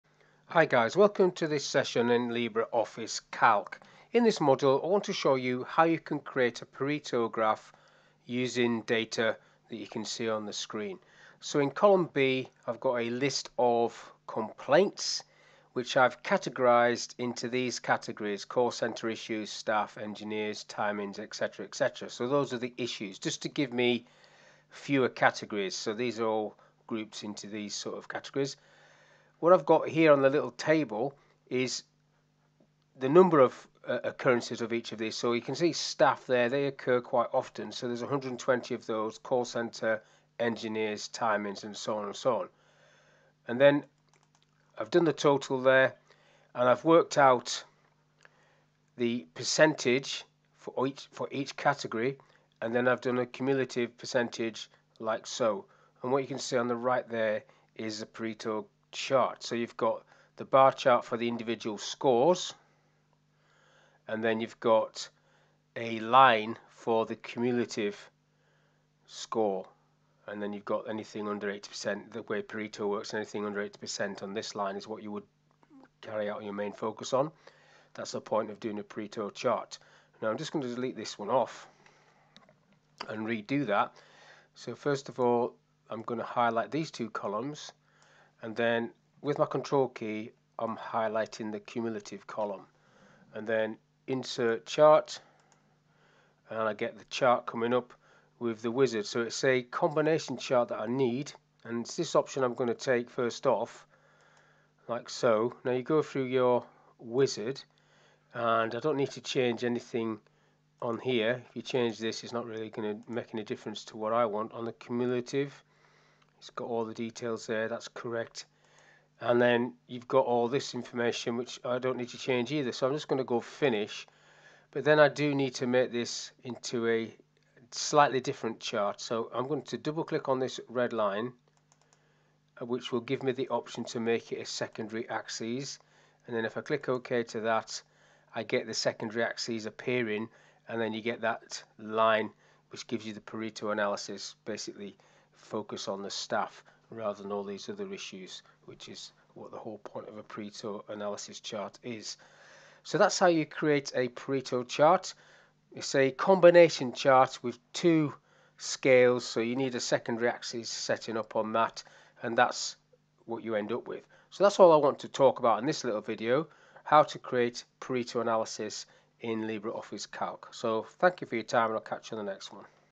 How to create a Pareto chart in LbreOffice Calc
This is a LibreOffice Calc tutorial covering, how to create a Pareto chart. The tutor starts of by explaining how the data table is arranged. The tutor then inserts a charts and selects the combination option. The tutor then covers how to create a secondary axis for the cumulative column. The chart displays a line and a column. LibreOffice calc. Calc tutorial. How to create a Pareto chart in LibreOffice Calc.